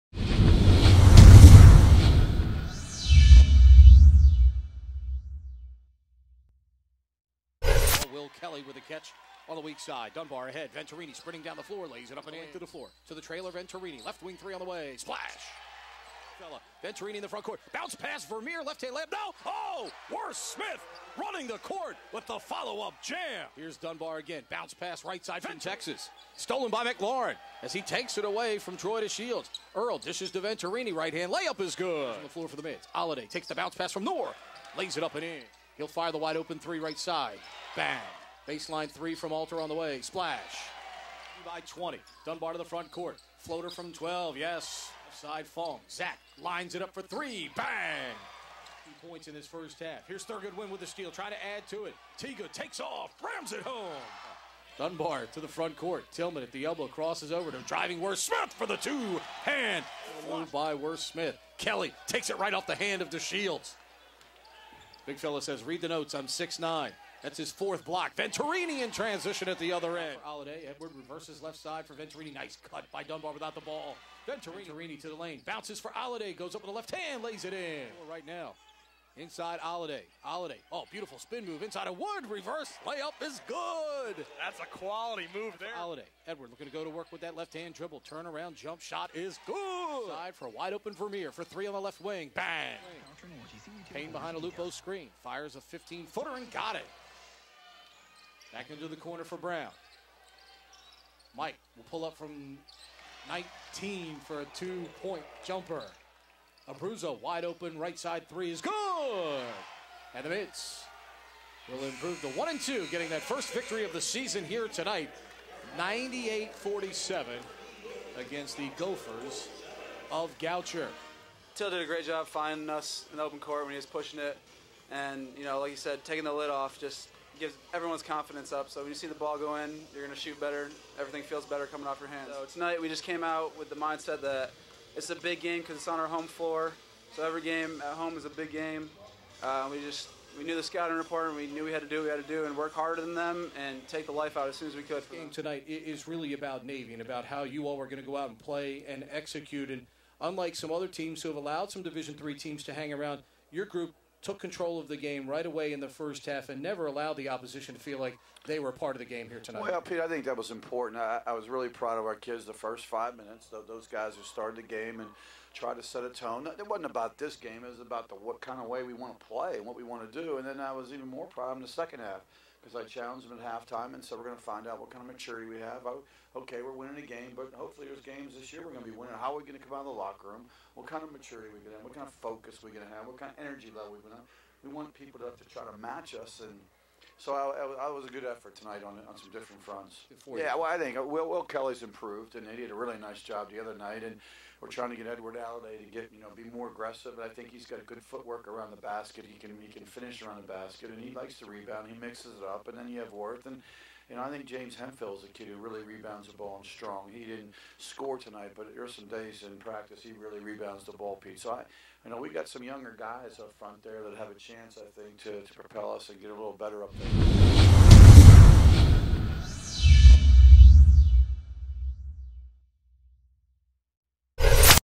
Navy Men's Basketball Highlights vs. Goucher
Highlights of Navy's 98-47 win over Goucher on Nov. 13, 2013 with interviews with Brandon Venturini and Ed DeChellis.